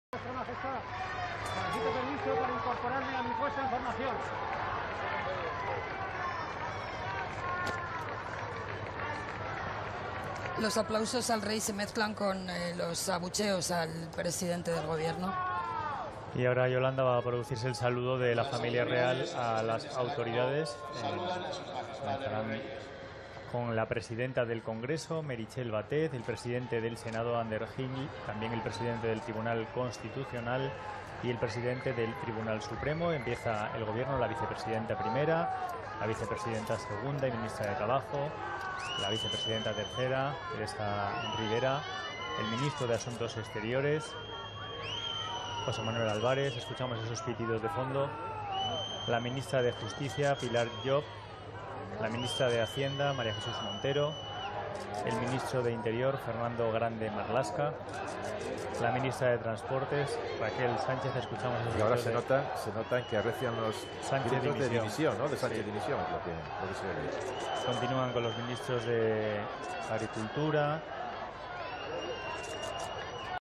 España: Abuchean a Sánchez y exigen su dimisión en el desfile militar del 12 de octubre
Abuchean a Sánchez y exigen su dimisión en el desfile militar del 12 de octubre